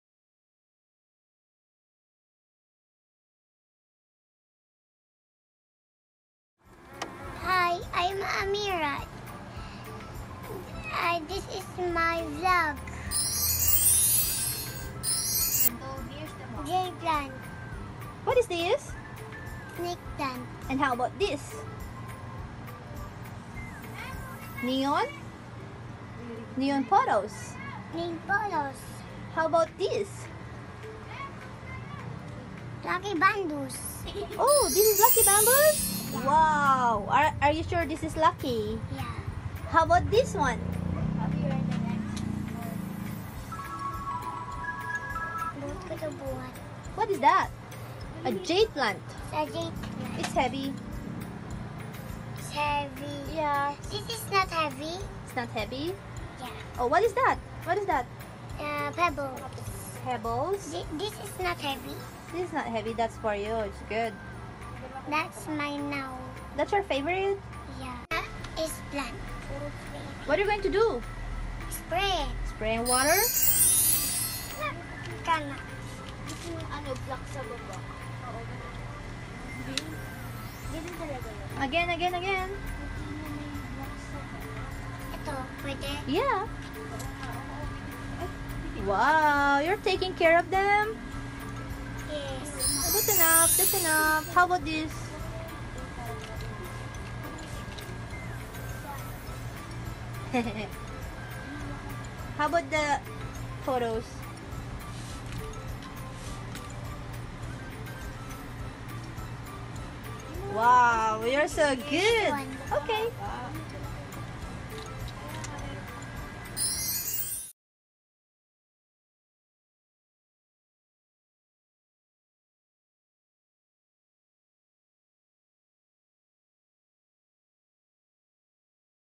My Ameers | Soil Sisters Ph
Let’s all introduce our kids to Garden!  Teach them about the environment.

Gardening is healthy and fun activity for children! Let’s encourage them to involve in planting and watering the plants!